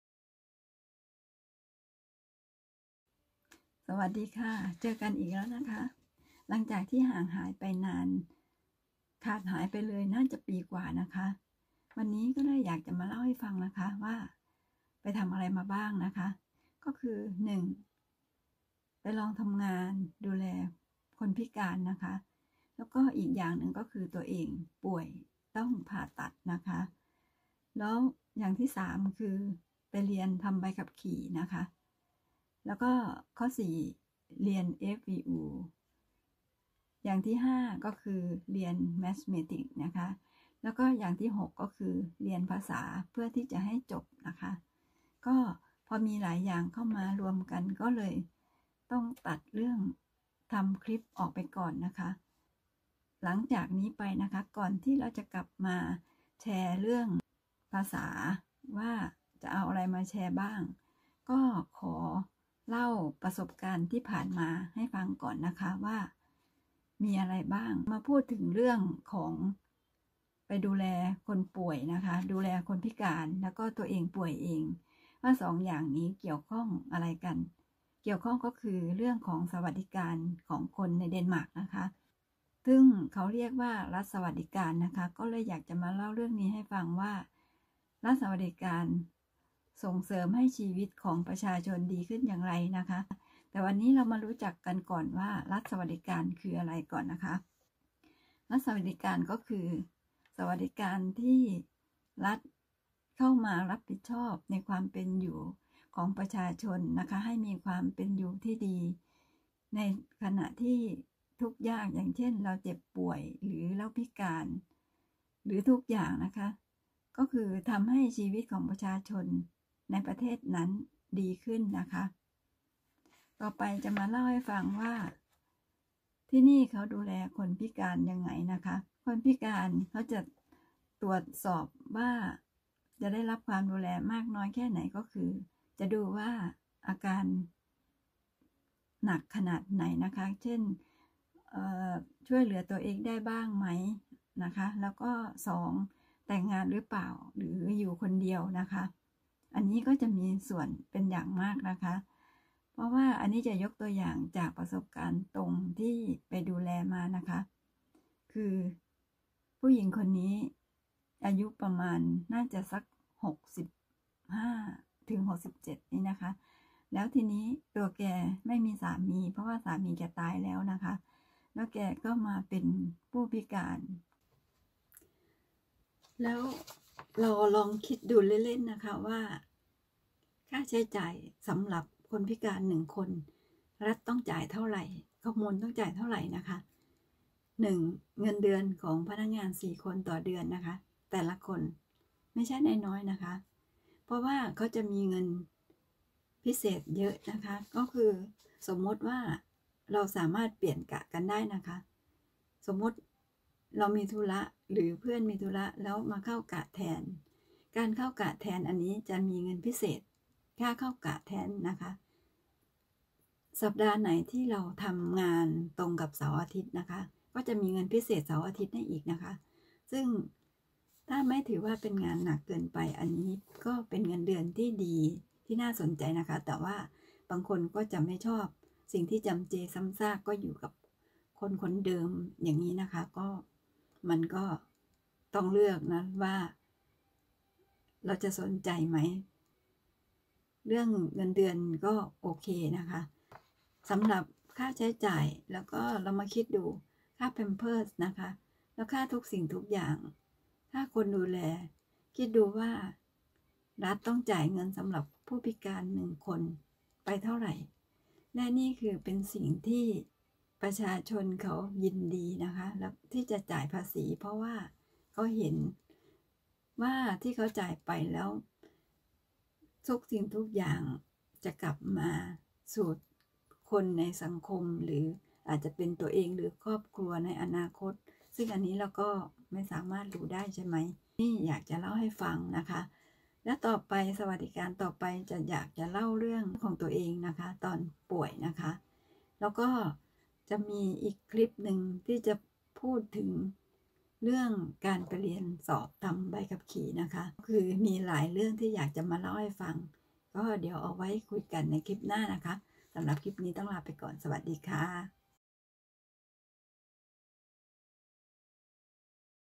รัฐสวัสดิการดีอย่างไร สำหรับการดูแลผู้พิการในเดนมาร์ก@Rak Achira Mashare
คลิปนี้นำเรื่องการดูแลบุคคลทุพลภาพ หรือ คนพิการในเดนมาร์ก
ว่าพวกเขาสามารถได้รับการดูแลดีอย่างไร
สำหรับคนที่อยู่คนเดียว ไม่มีคู่สมรส ฟรี เกือบ100%
(เพราะอาจจะมีบางอย่างที่ต้องจ่าย )
ถ้าอาการที่ไม่สามารถช่วยเหลือตัวเองได้
มีคนดูแลตลอด24ชั่วโมง และ
มีอีกหลายๆอย่างที่รัฐดูแลดีแบบเหลือเชื่อค่ะ
ฝากติดตามด้วยนะคะ ซึ่งจะนำสาระดีมานำเสนอต่อๆไปค่ะ
ซึ่งสามารถติดตามได้ ที่นี่ Rak Achira Mareshare